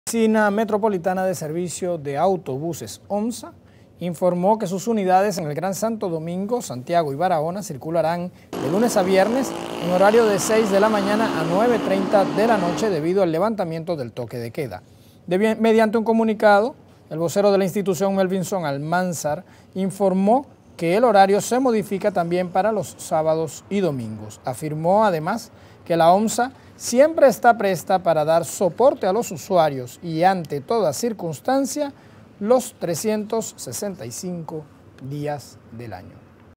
Autobuses OMSA circularán en nuevos horarios desde este domingo
La Oficina Metropolitana de Servicios de Autobuses, OMSA, informó que sus unidades en el Gran Santo Domingo, Santiago y Barahona, circularán de lunes a viernes en horario de 6 de la mañana a 9:30 de la noche, debido al levantamiento del toque de queda.

Mediante un comunicado, el vocero de la institución, Melvinson Almánzar, informó que el horario se modifica también para los sábados y domingo.